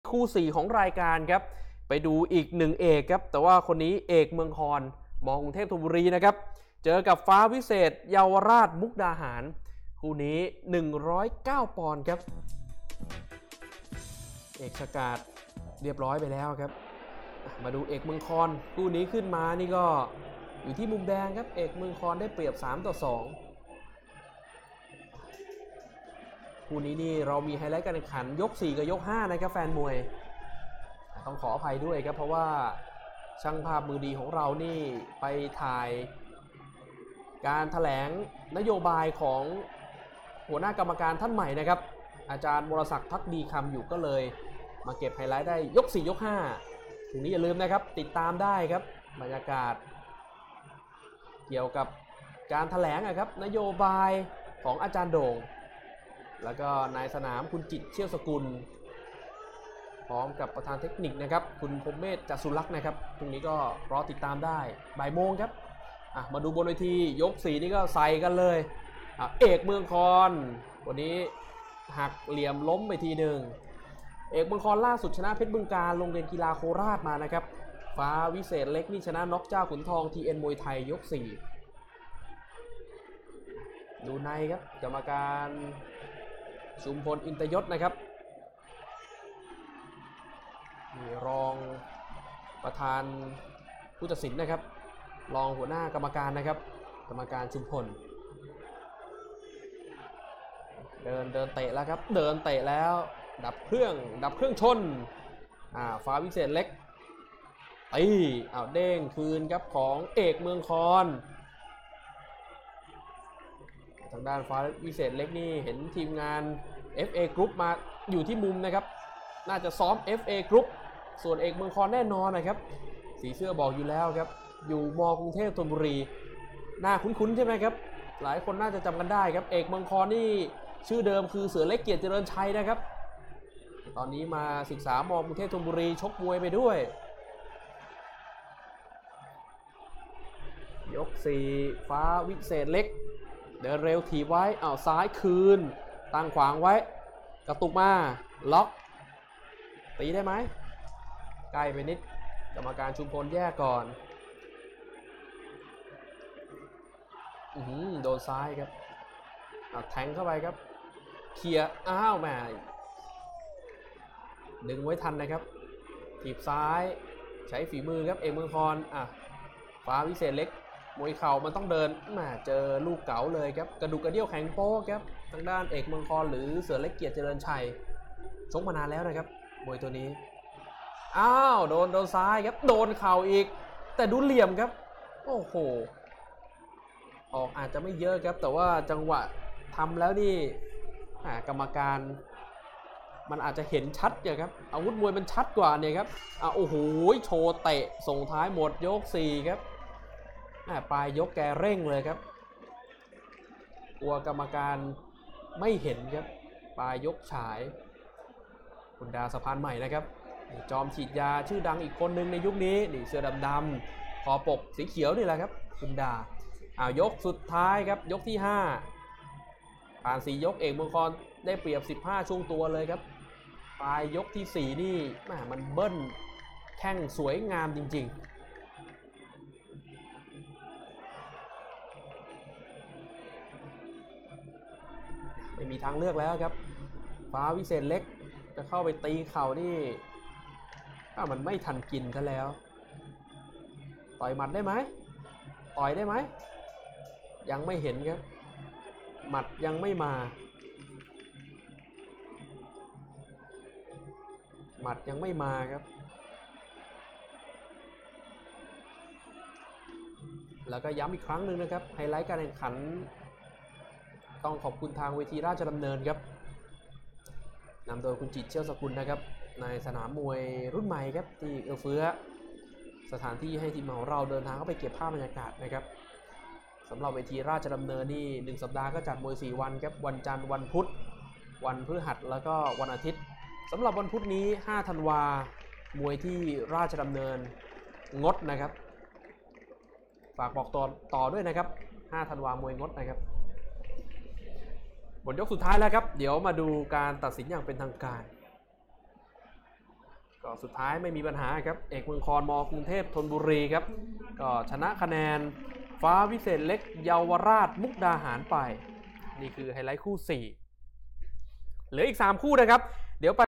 คู่ 4 เอกเมืองคอน ม.กรุงเทพธนบุรี - ฟ้าวิเศษเล็ก เยาวราชมุกดาหาร (AekMueangKhon VS FahWisetLek)
มวยไทย ศึกชูเจริญมวยไทย ที่ เวทีราชดำเนิน
(Battle of Chujaroen Muaythai at Rajadamnern Stadium)

วันจันทร์ที่ 3 ธันวาคม 2561
(Monday 3 December 2018)

ผู้จัด : ชูเจริญ รวีอร่ามวงศ์
(Promoter : Mr. ChuJaroen RaweeAramwong)

คู่ 2 จอมโว ศิษย์เชฟบุญธรรม - ยอดสามเอ ดาบเชษฐ์สันติบาล (JomWo SitChefBunTham VS YodSamAe DabDhetSantiban)

คู่ 3 เอกฉกาจ ม.กรุงเทพธนบุรี - กิ่งพลอย นักสู้ยิม (AekChakat BangkokThonBuri University VS KingPloy Naksu Gym)

คู่ 4 เอกเมืองคอน ม.กรุงเทพธนบุรี - ฟ้าวิเศษเล็ก เยาวราชมุกดาหาร (AekMueangKhon BangkokThonBuri University VS FahWisetLek YaoWaratMukdahan)

คู่ค้ำ เดือน ก.กัมปนาท - ฟ้าส่องแสง หนุ่มพรเทพ (Duean Ko.Kampanat VS FahSongSaeng NumPhonthep)

คู่รอง คชสาร ว.วิวัฒนานนท์ - ชลสิทธิ์ เกียรติพูลผล (KhotChasan Wo.WiwatNanon VS ChonSit KiatPhunPhon)

คู่เอก อีที เพชรสมนึก - เพชรงาม เกียรติพูลผล (ET PhetSomNuek VS PhetNgam KiatPhunPhon)